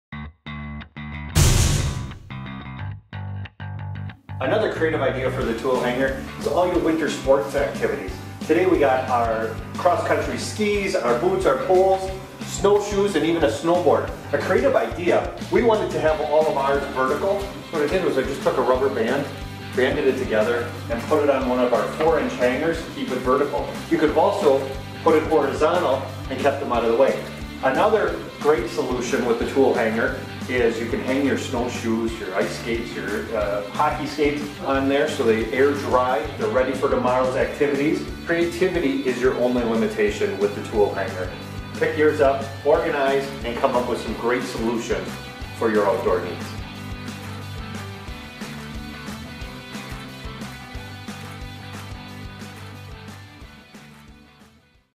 ToolHANGER Winter Sports
The ToolHANGER is a new plastic organization board with twist and locking hangers to organize, display and hang your tools. The ToolHANGER has six unique twist and locking hangers designed to hold your sockets, wrenches, pliers, hammers, screwdrivers, power drills, shovels, brooms, hunting and sporting equipment, kitchen utensils, crafting supplies and more.